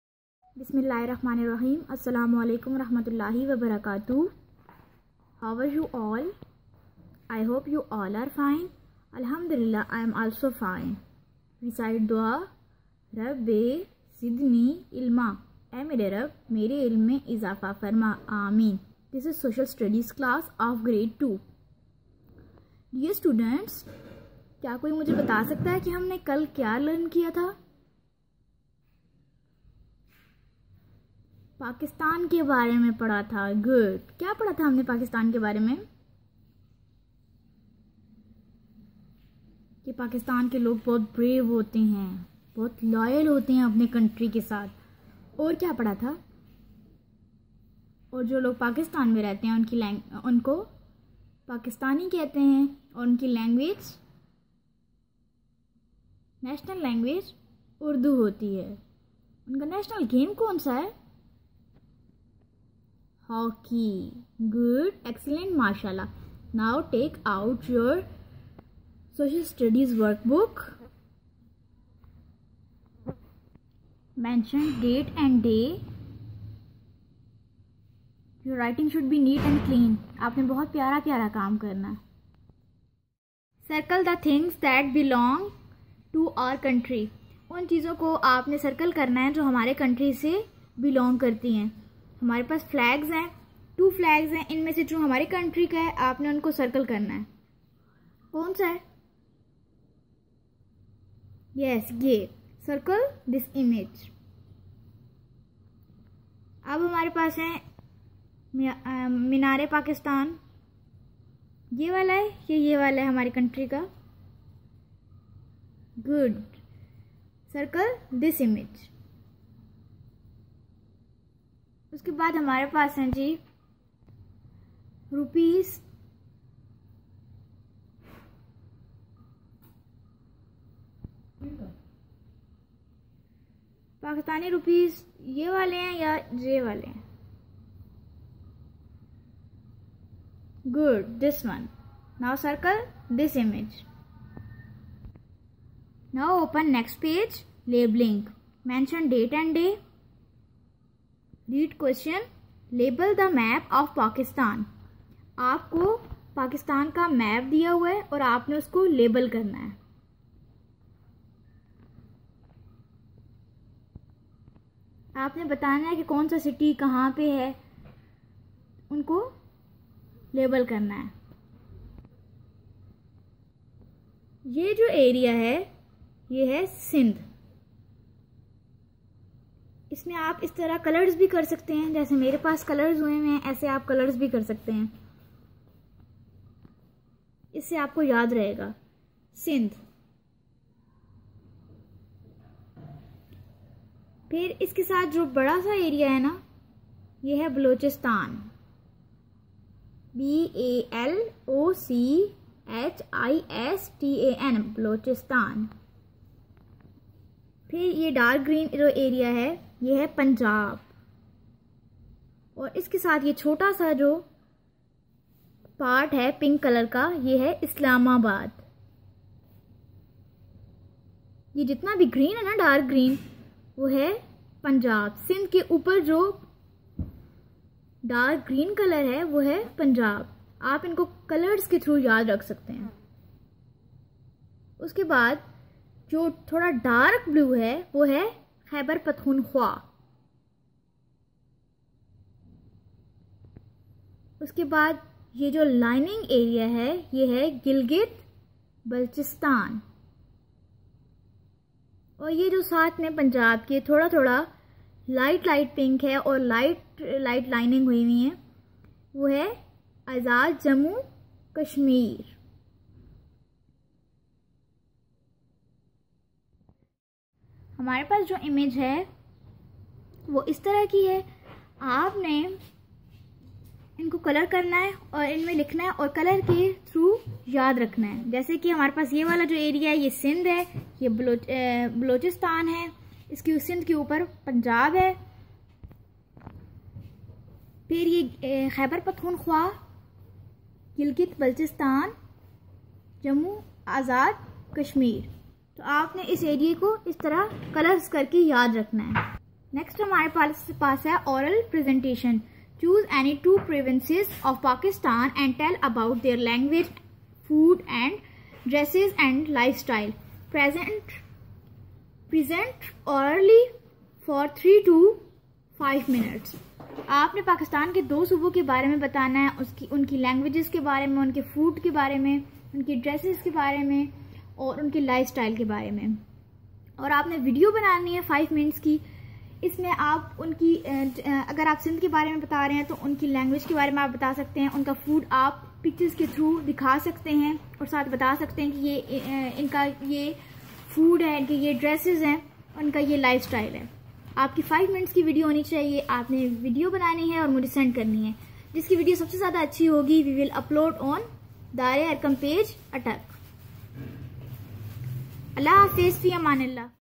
Class#17 of Social Study for grade 2 students (My Interpretation) DareArqam Schools
AOA Hoping for your best of Health and Emaan.
This is Class # 17 of Social Studies for Grade 2 students.
Lesson 4
Topic: Exercise on Work Book
  Circle the thins that belongs to our country Pakistan.

Label the map of Pakistan.

Oral presentation.

Islamic Republic of Pakistan

Matching the columns

Circle the correct answers and
Answer the questions.
When did Pakistan come into being?
Write down the names of five major rivers of Pakistan.
1. Jhelum.
2. Chenab.
3. Ravi.
4. Sutlej.
5. Indus.

Who was the founder of Pakistan?
Quad-e-Azam Muhammad Ali Jinnah
How many provinces of Pakistan?

Karachi is the biggest city of Pakistan.
Urdu is the national language of Pakistan.
Hockey is the national game of Pakistan.
Five major rivers of Pakistan.

DareArqam School
SocialStudy for grade 2 students Class#16 DareArqam Schools